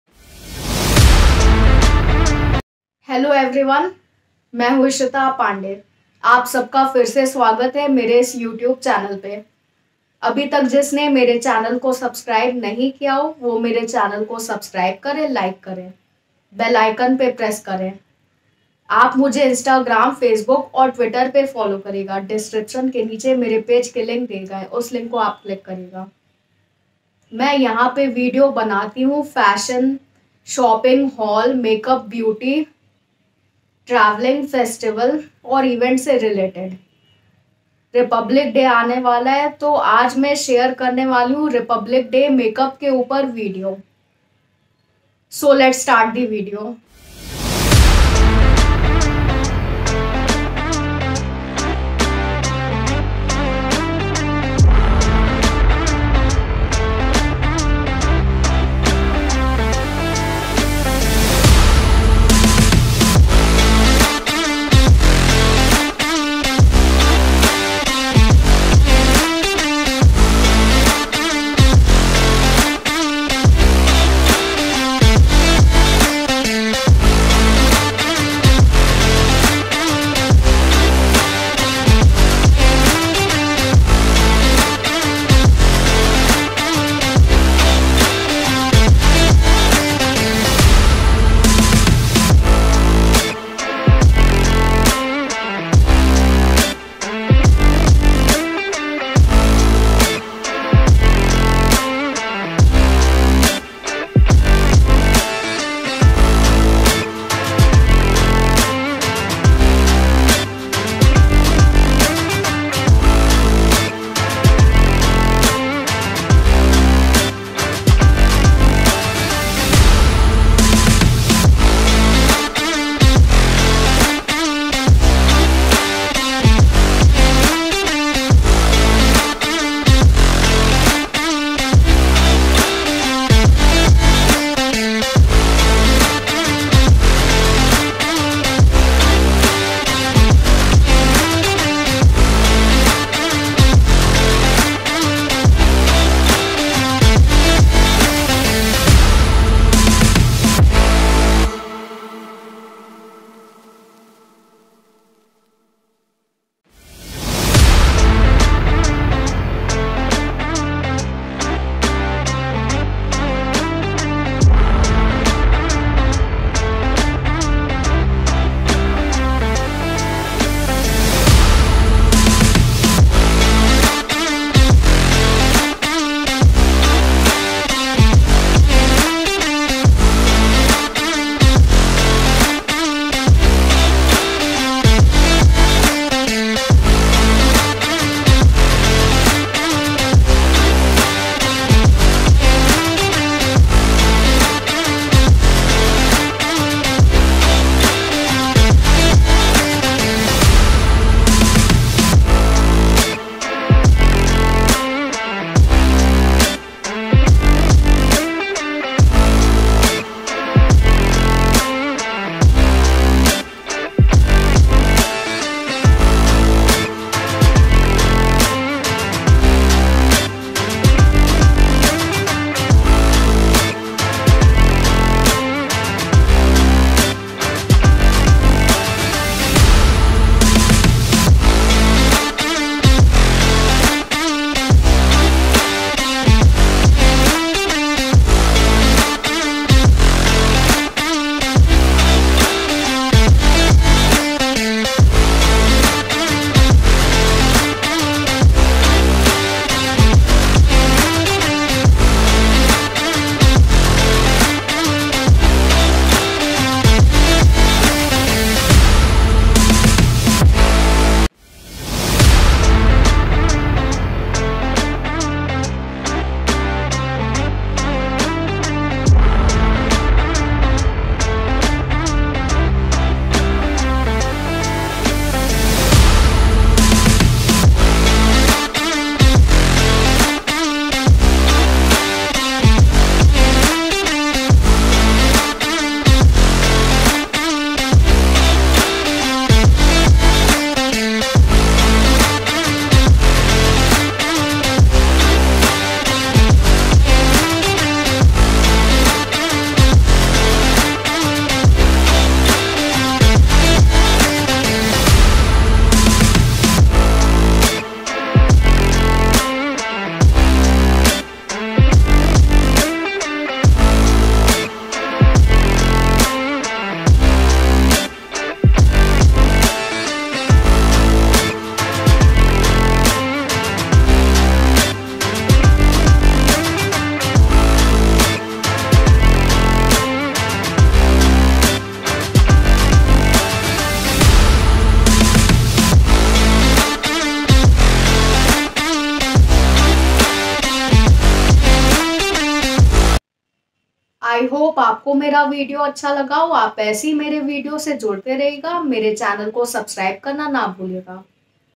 Republic Day Look 🇮🇳 || Makeup Tutorial 💄 || Creativity ❤️ || Vlog ✨
Hello everyone, this is Ishita Pandey. Welcome back to YouTube Channel, today I will share Republic Day's Makeup Video.